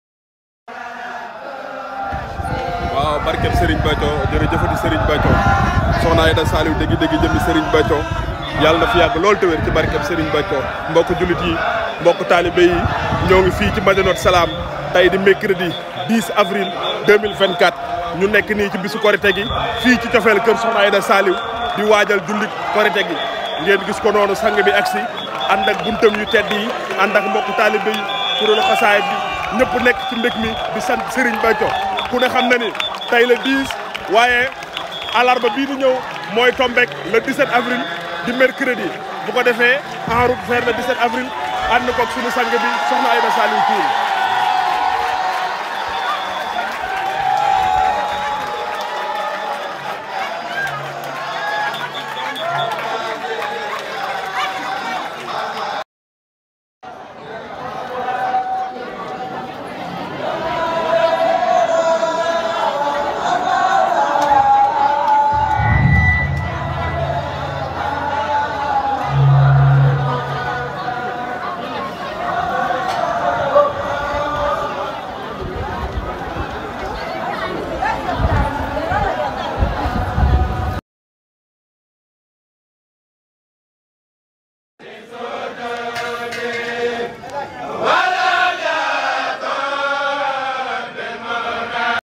KORITÉ 2024:L'ARRIVÉE MAJESTUEUSE DE SOKHNA AIDA SALIOU THIOUNE
Célébrez la Korité 2024 avec nous en regardant l’arrivée majestueuse de Sokhna Aida Saliou Thioune. Cette vidéo capture l’essence de la célébration et l’aura impressionnante de Sokhna Aida. Rejoignez-nous pour vivre ces moments inoubliables.